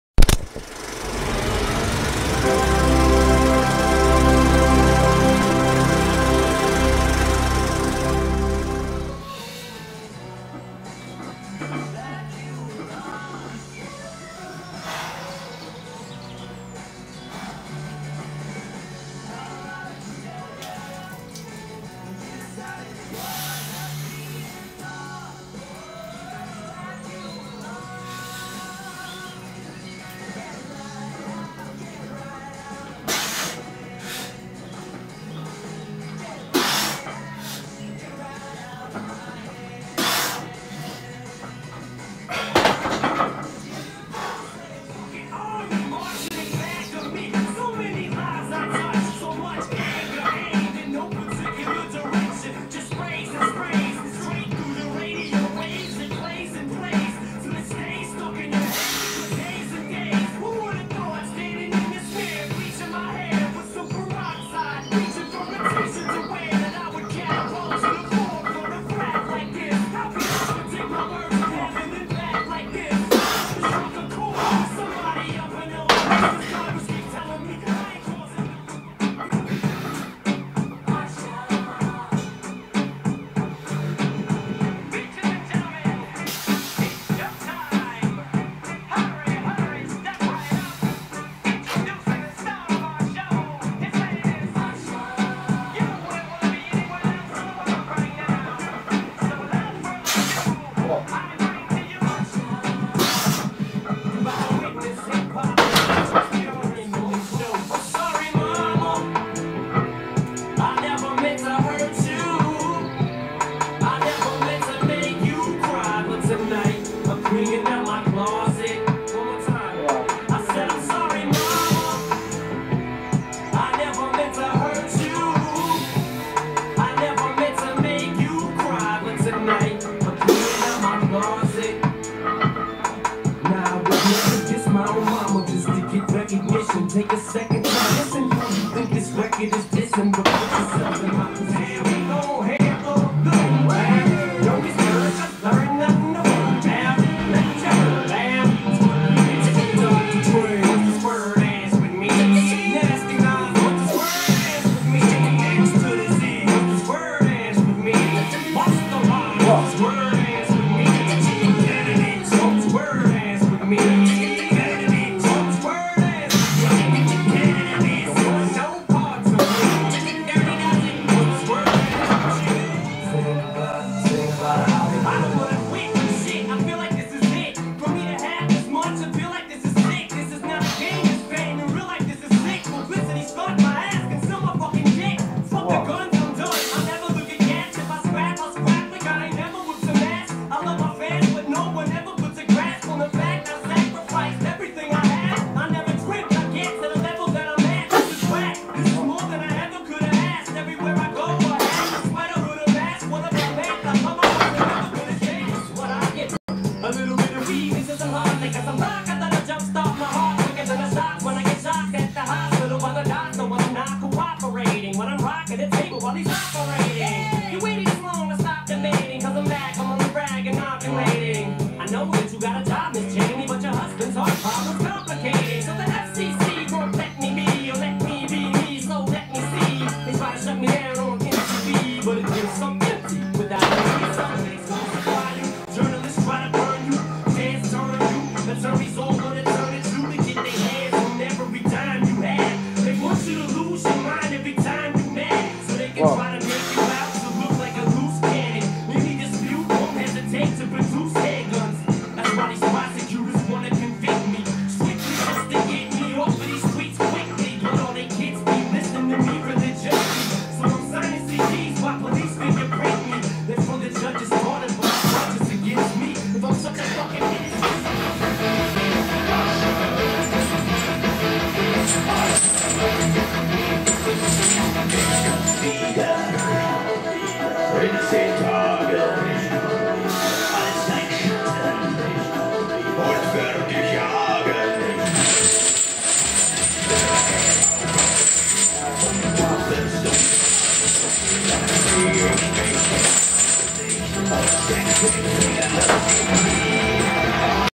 Beltess Squats - 165kg x 3 x 8 - Squats With Chains - 165kg x2 - 08.06.2014
First squat session after my holiday - Felt a little rusty and I was struggling to breathe :)